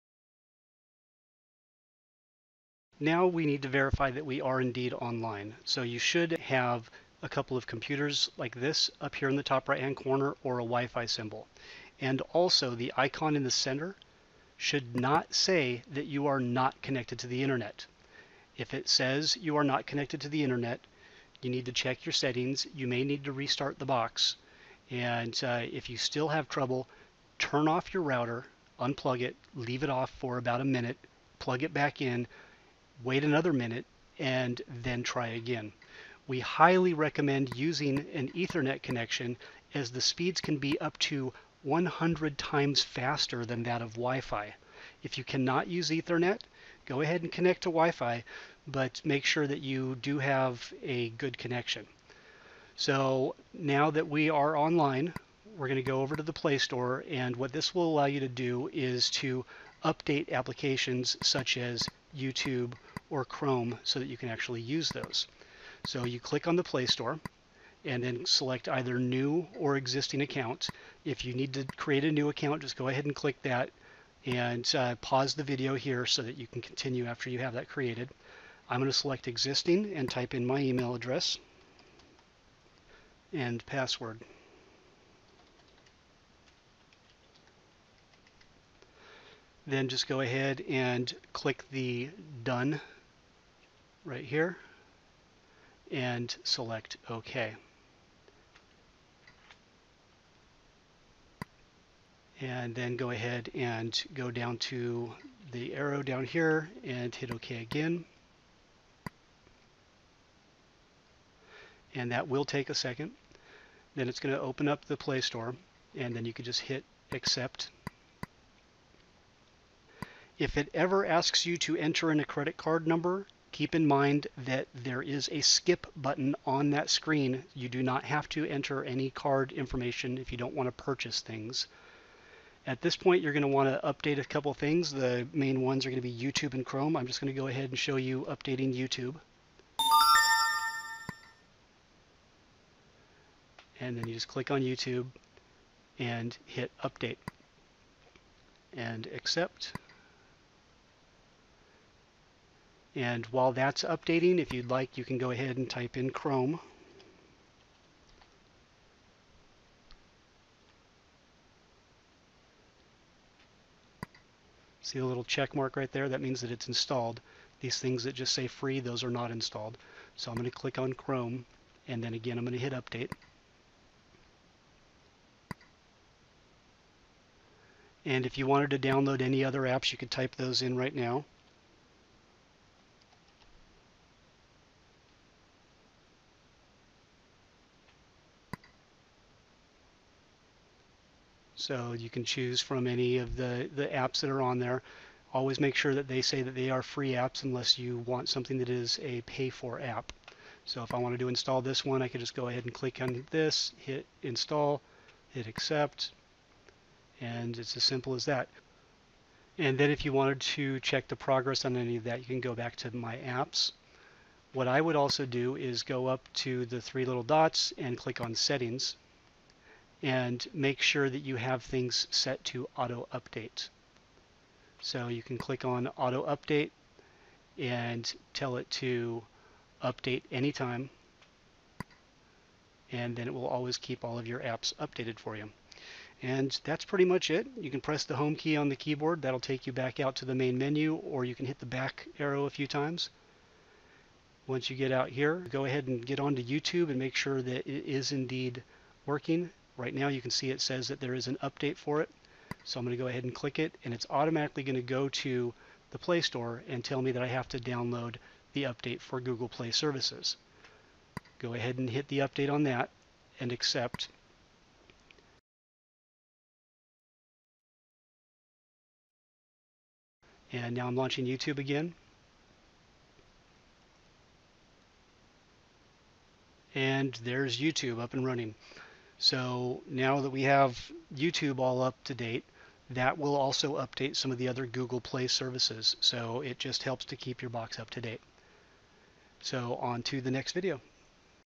Getting Started - Google account for Updates - StreamLine TV
Getting Started - Setting up your Google account for Updates and access to the Play Store - StreamLine TV